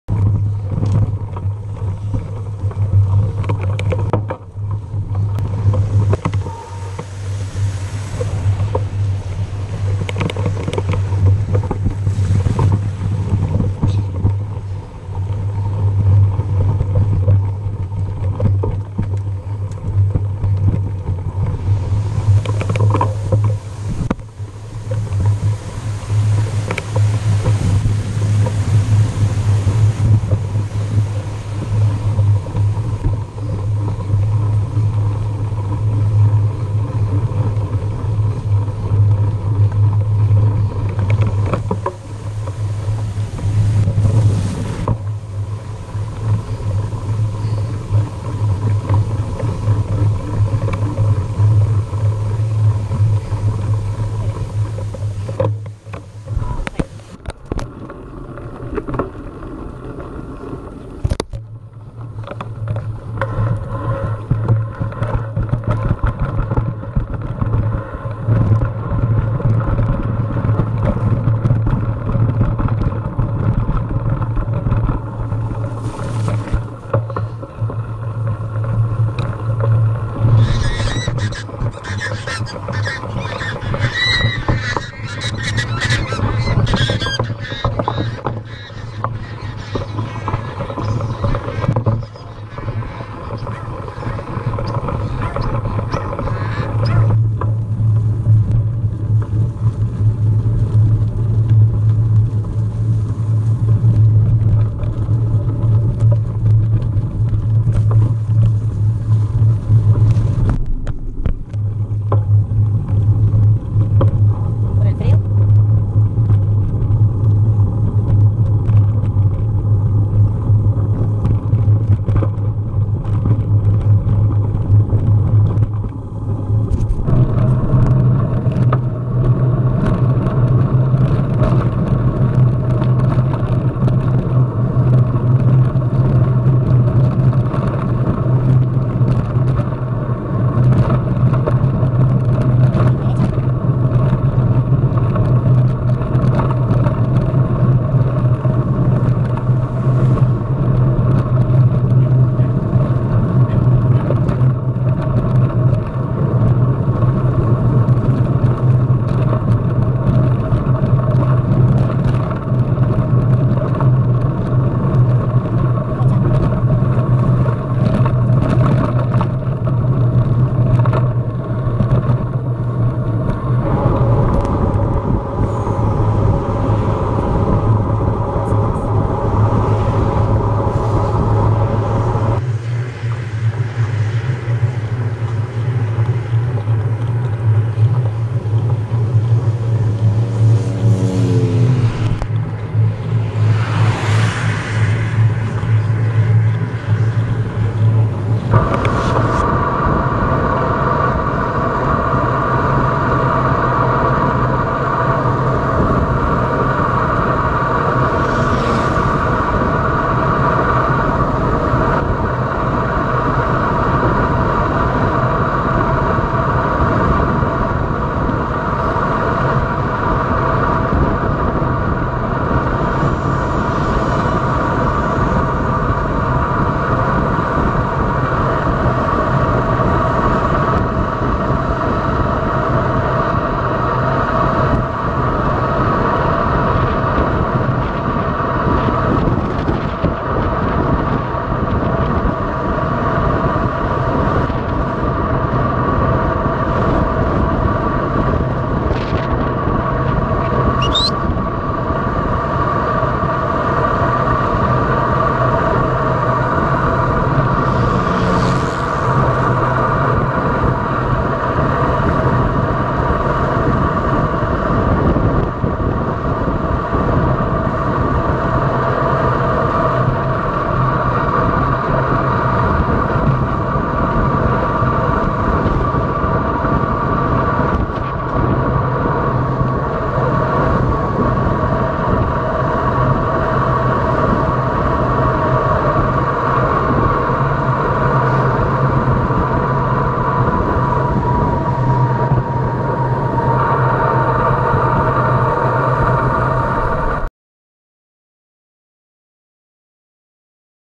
Entre Cumbres MTB la Argentina
Ruta mtb por la antigua vía a la Vega